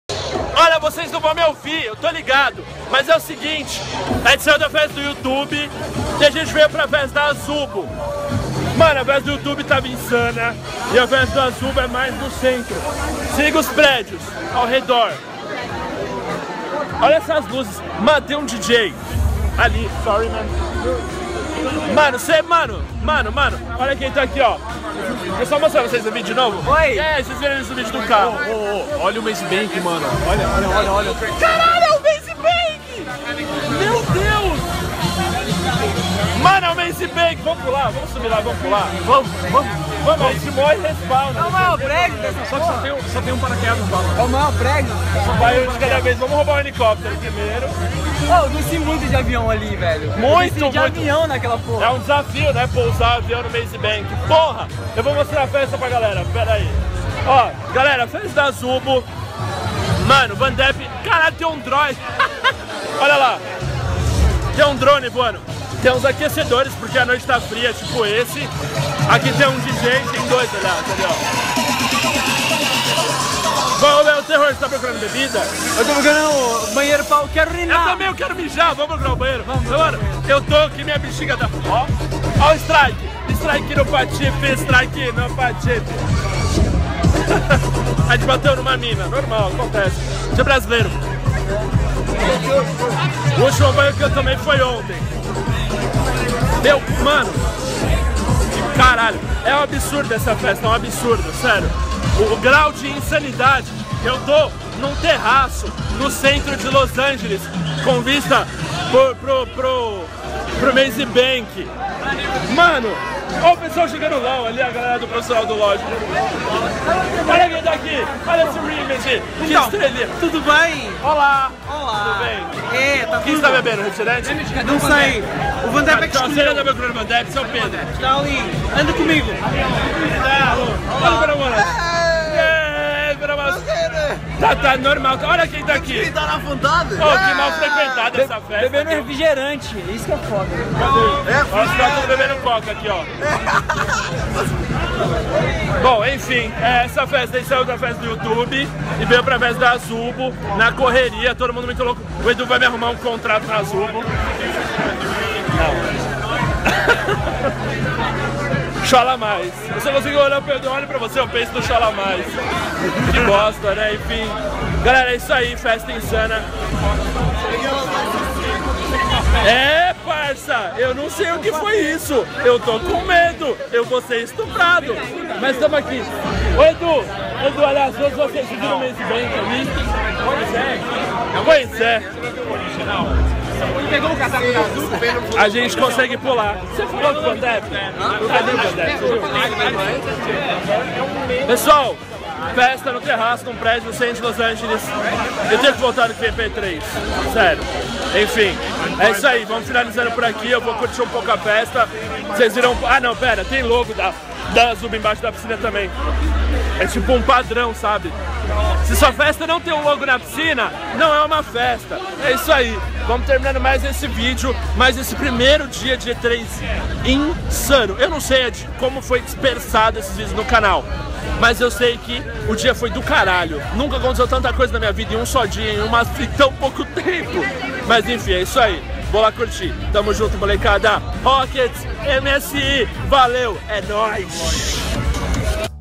A FESTA MAIS INCRÍVEL QUE JÁ FUI! Drones, Maze Bank, Terror, Funky, Edu, Remedy, Feromonas e mais!
Os grandes responsáveis por eu estar realizando o SONHO de estar na E3 são a MSI e a Rocketz: CONFERE

Valeeu!